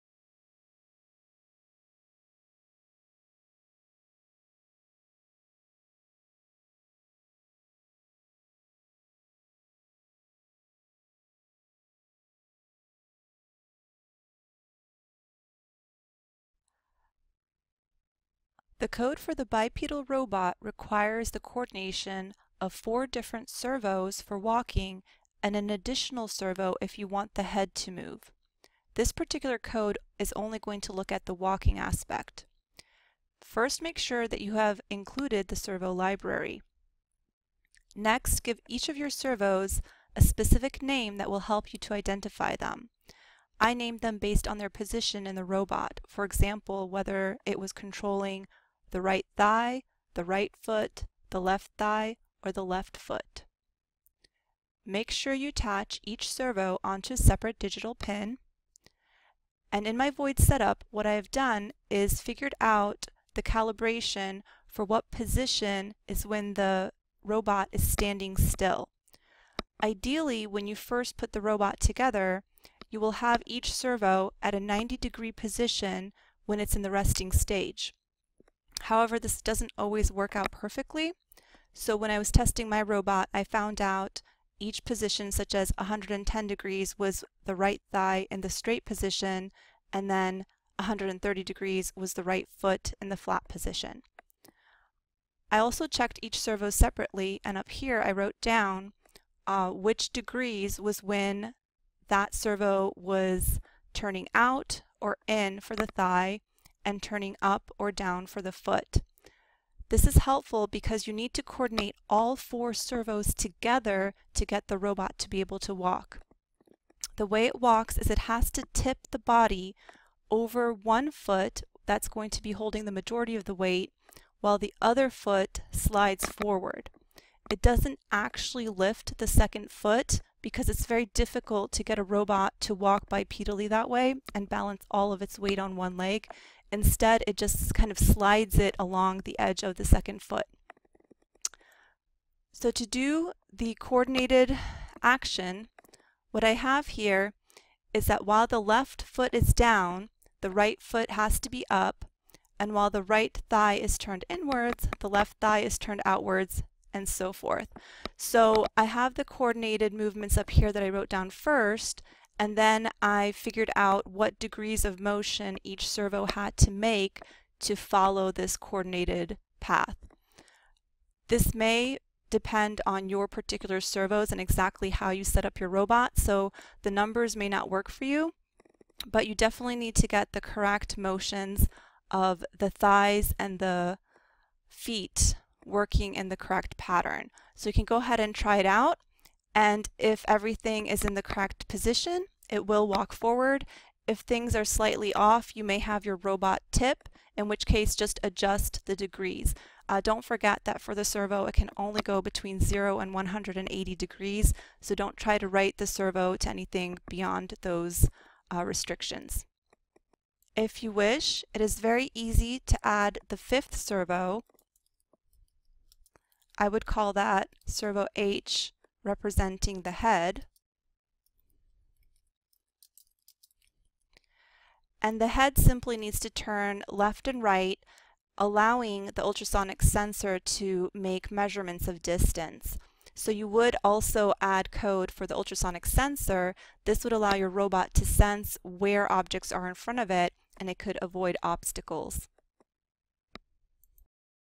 Arduino 5e bipedal robot
How to get a bipedal robot to walk using servos.